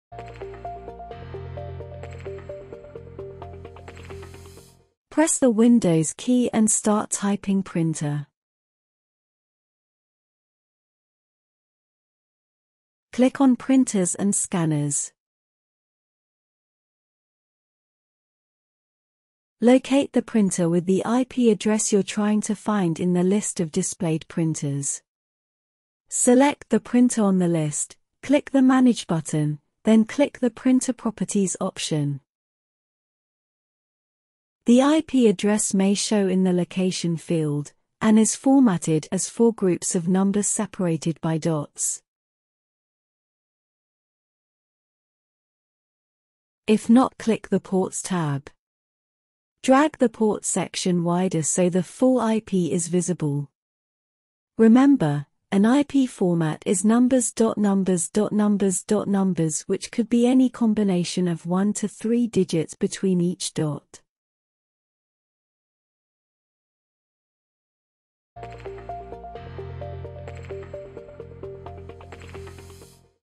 Sharp OneStop Guide: Get an IP for a printer
This video guides you through how you can find the IP address for a specific printers within your fleet
 It will take you a step by step process from start to finish.